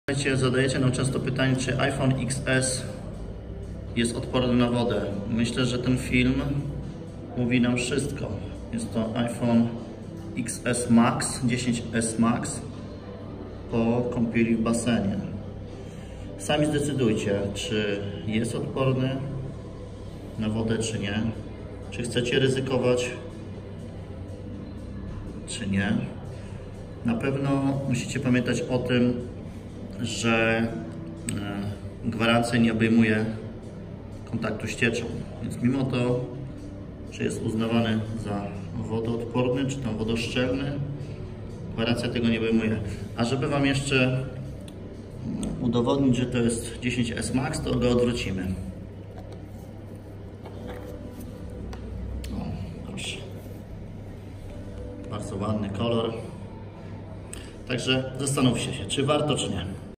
Czy iPhone Xs Max jest wodoodporny wodoszczelny? iS iPhone Xs water resistance? ZbitaSzybka.pl
Pamiętajcie, że ZbitaSzybka naprawia zalane telefony.

iPhone Xs is not waterproof or water resistant